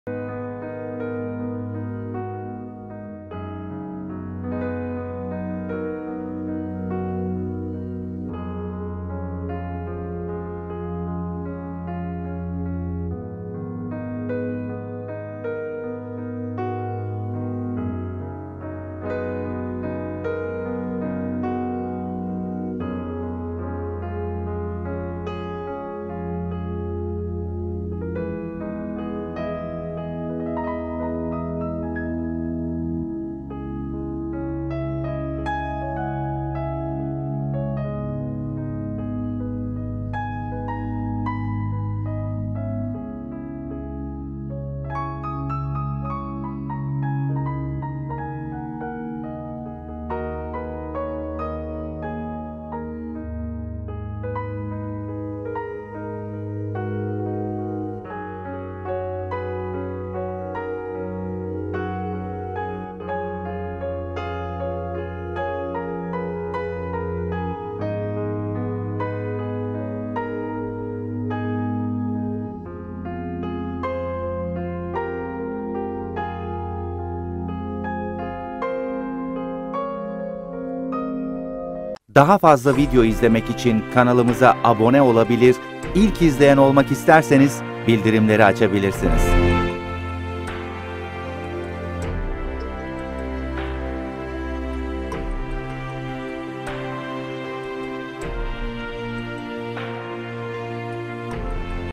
Amanoslar Sonbahar Renkleriyle Görenleri Mest Ediyor
Hatay'ın Hassa ilçesi Amanos Dağları eteklerindeki meyve bahçeleri, sonbahar renkleriyle kartpostallık manzaralar sunuyor.